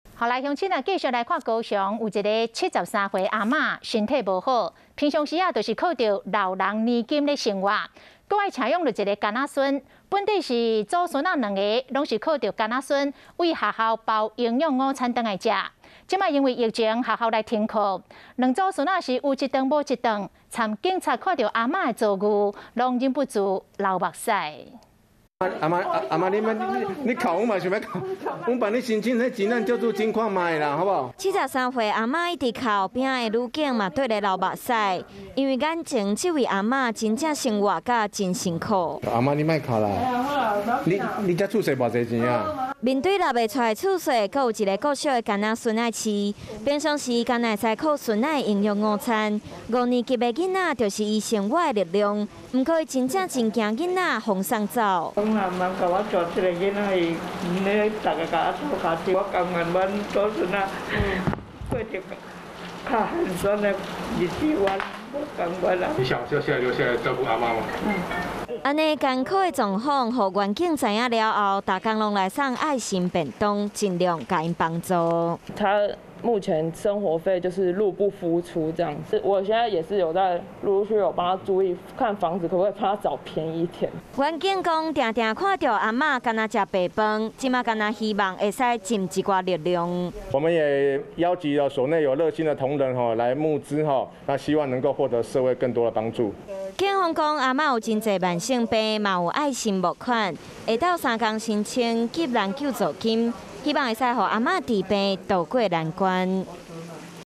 祖孫因疫情停課沒便當 暖心警送餐｜華視台語新聞 2021.07.23
高雄一名73歲老婦人身體不好，平時靠老人年金生活，還要獨立扶養一名曾孫，祖孫倆以往靠孩子從學校打包回來的營養午餐果腹，現在因為疫情學校停課，祖孫倆變的有一餐沒一餐，轄區員警發現後，到阿嬤家裡送便當，而阿嬤的慘況也讓警察忍不住哭了。